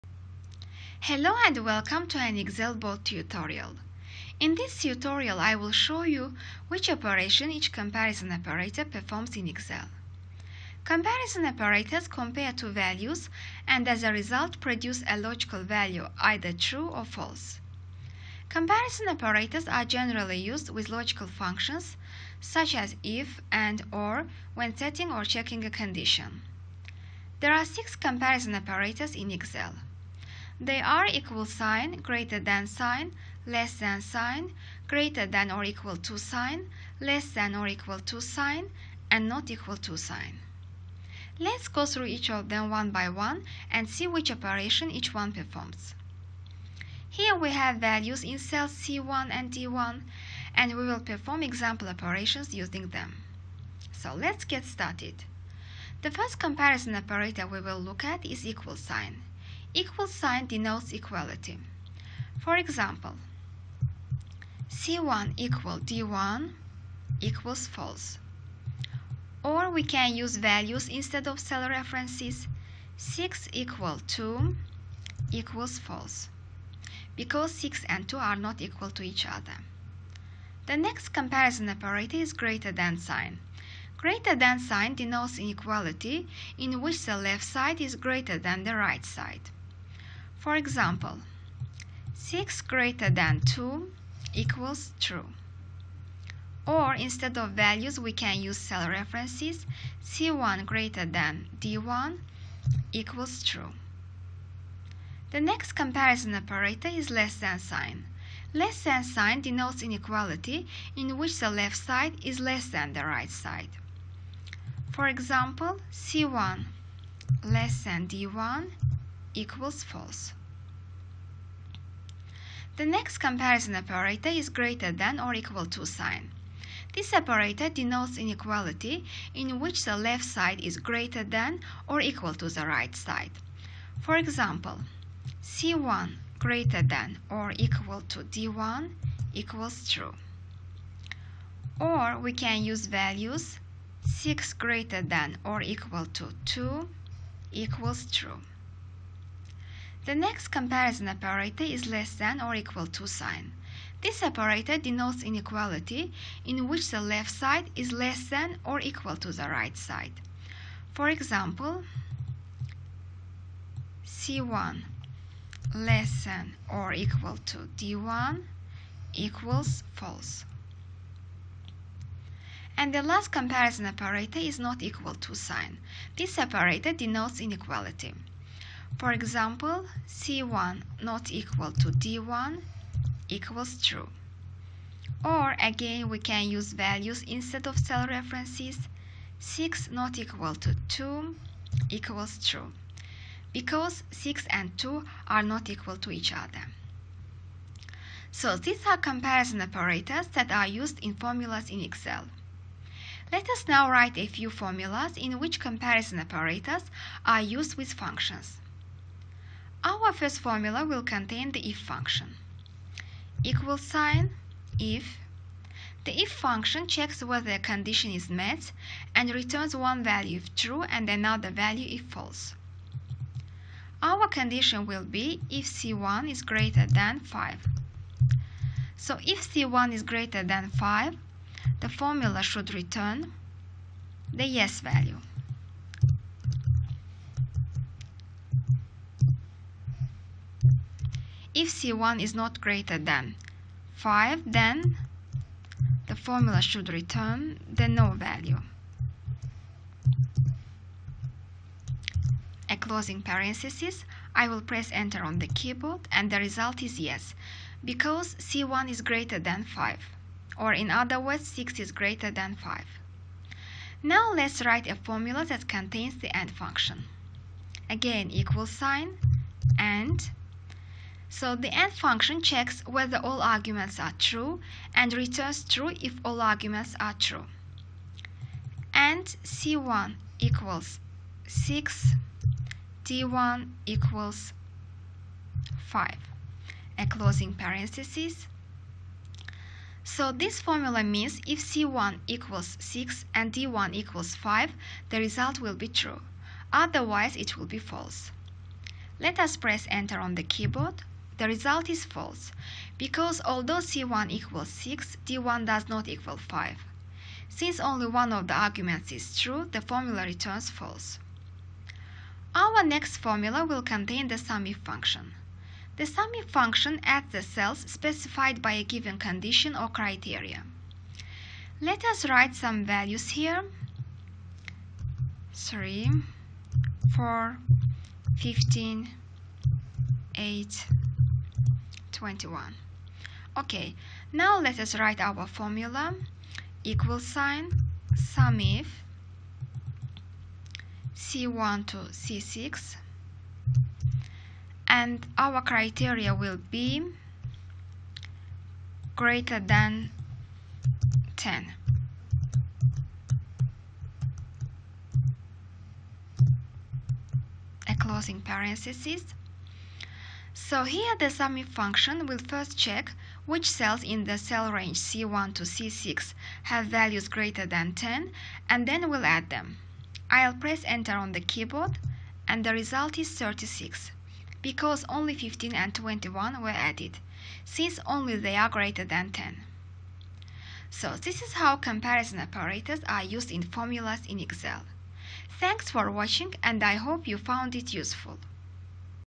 Comparison Operators in Excel - Easy Explanation of Each Operator with Examples
Comparison operators in Excel compare two values, and as a result, produce a logical value either TRUE or FALSE. They are generally used with logical functions (IF, AND, OR, etc.) when setting or checking a condition. There are 6 comparison operators in Excel: equal sign, greater than sign, less than sign, greater than or equal to sign, less than or equal to sign and not equal to sign.

In this tutorial, you'll learn about comparison operators in Excel and see how they are used in formulas.